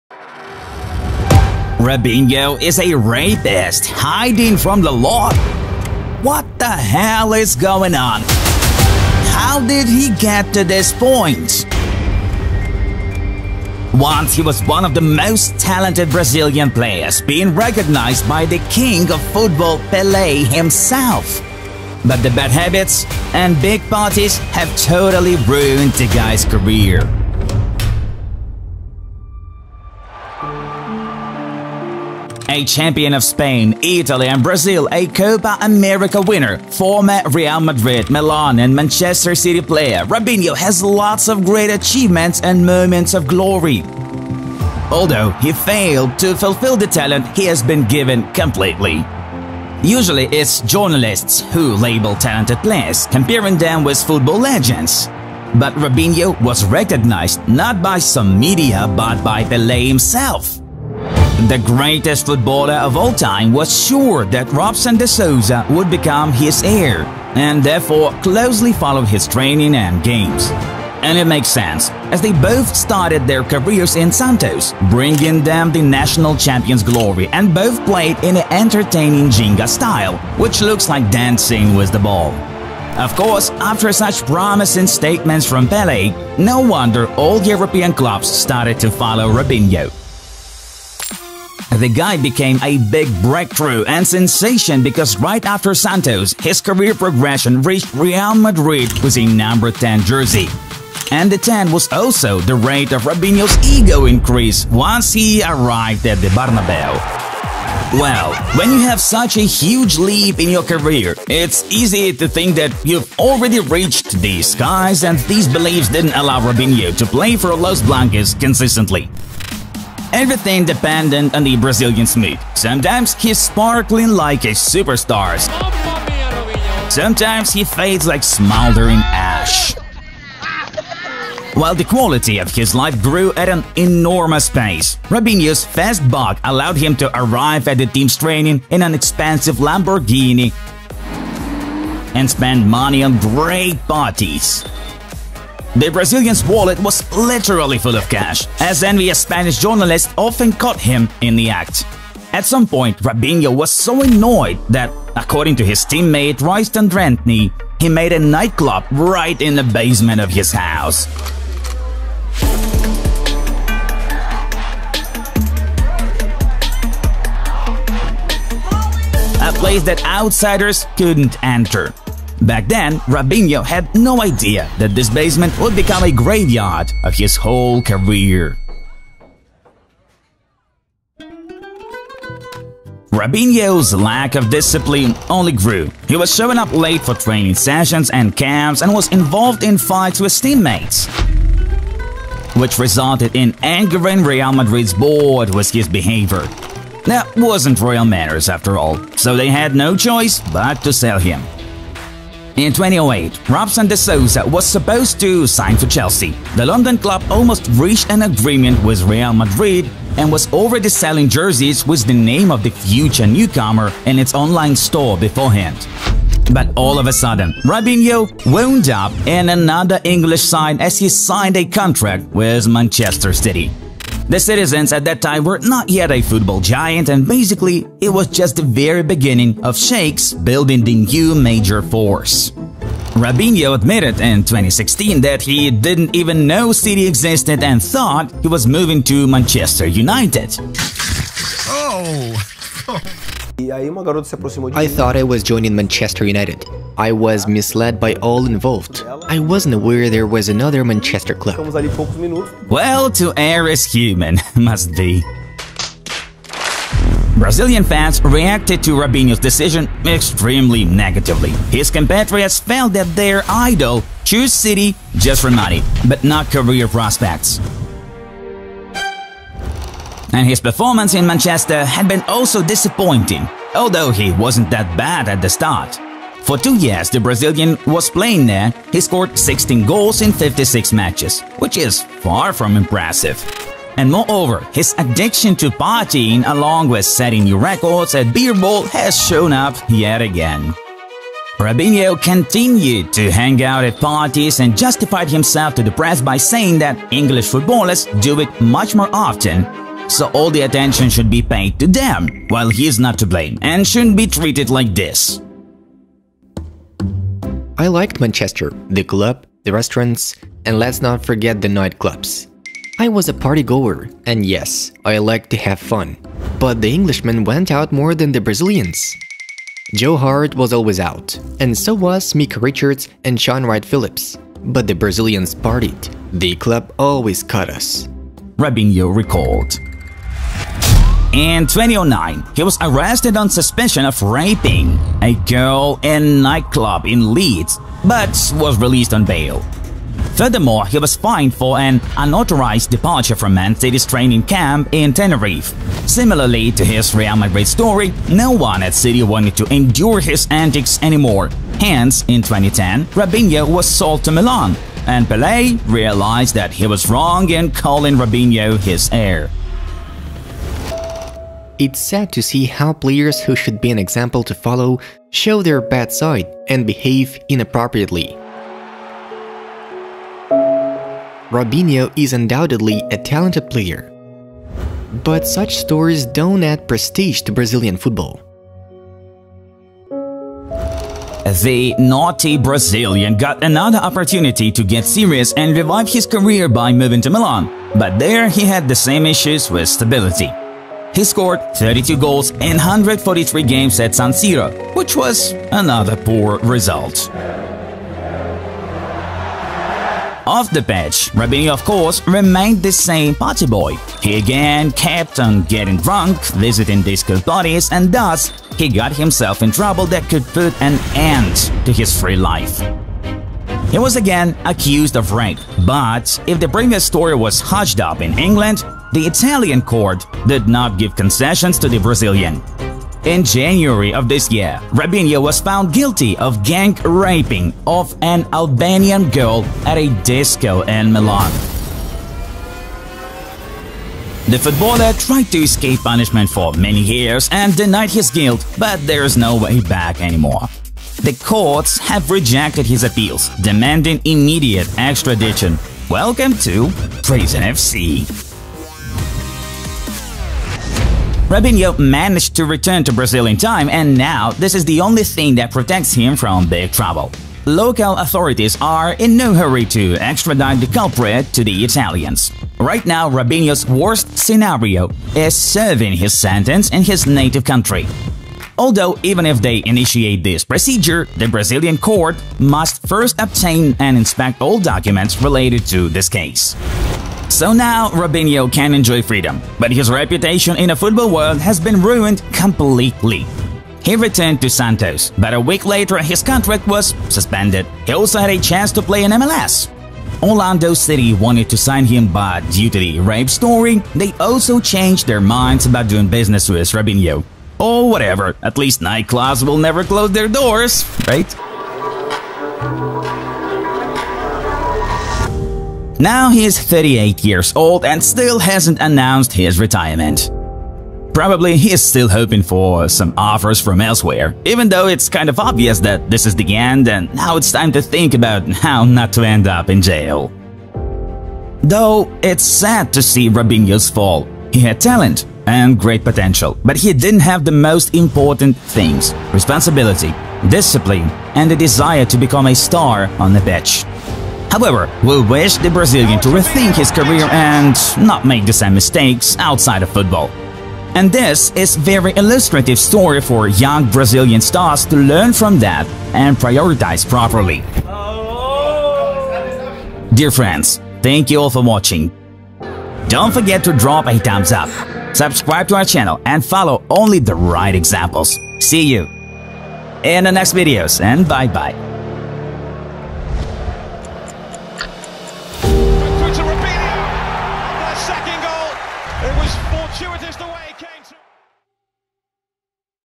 ROBINHO - From The “Next Pele” To Prisoner! What Happened To ROBINHO?
Robinho is a rapist hiding from the law? What the hell is going on?! How did Robinho get from the ''New Pele'' to prisoner?! Bad habits and big parties have totally ruined the guy’s career!

Welcome to the channel of exciting football stories full of both drama and positivity! 🎭